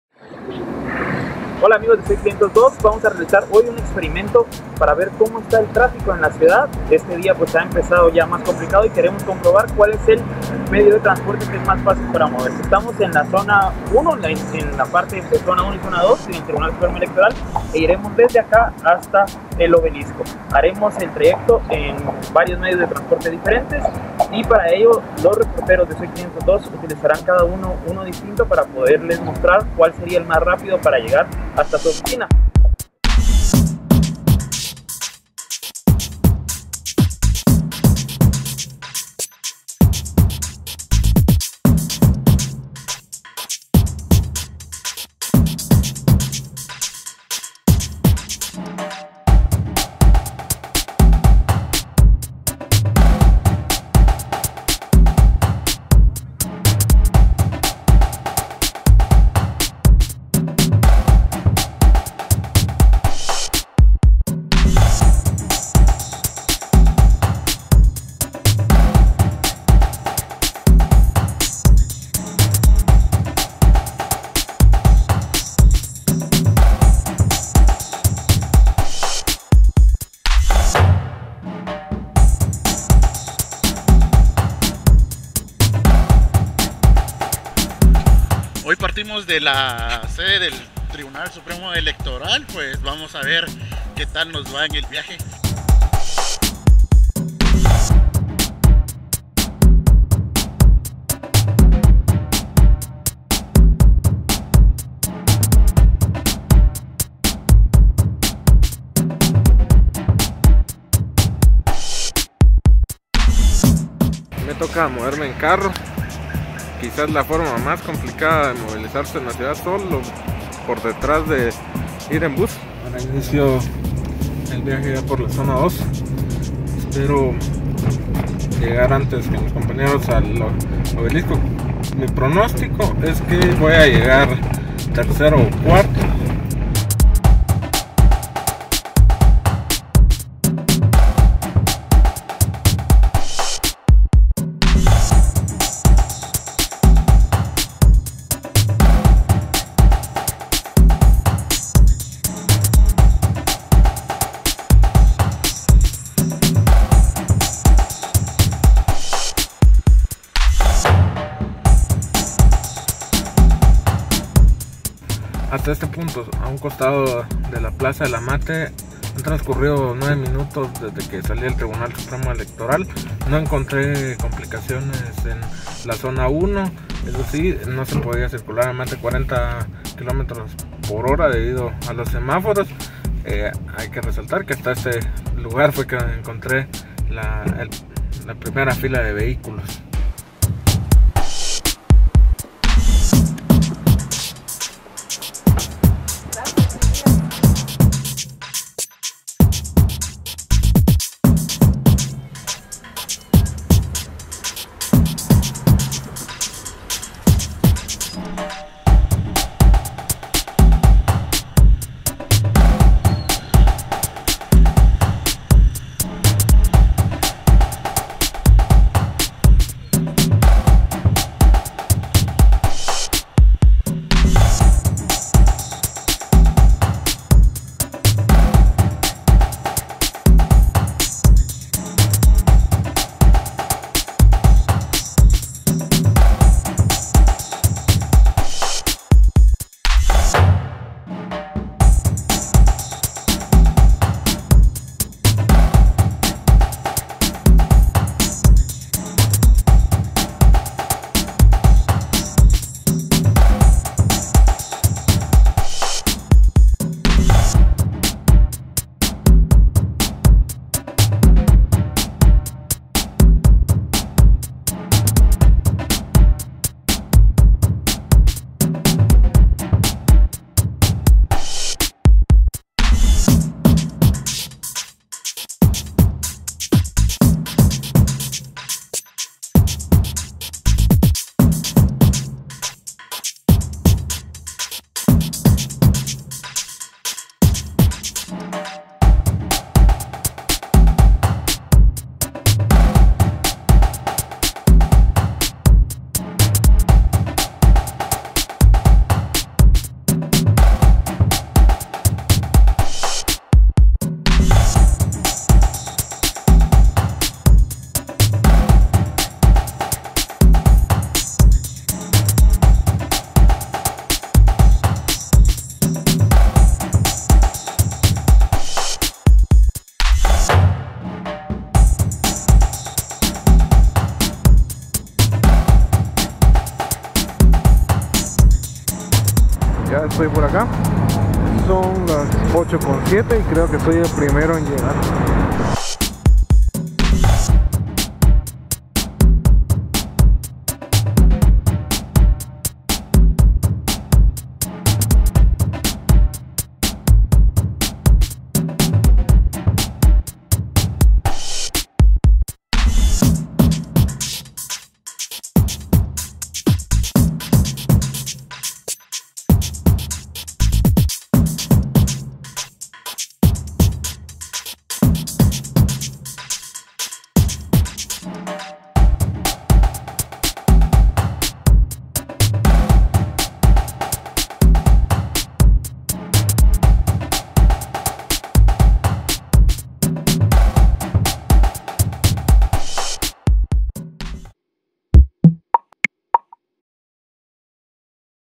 Experimento502: ¿Qué medio de transporte es más efectivo?
Un grupo de reporteros de Soy502 salió a las calles para realizar un experimento de la movilización en la ciudad.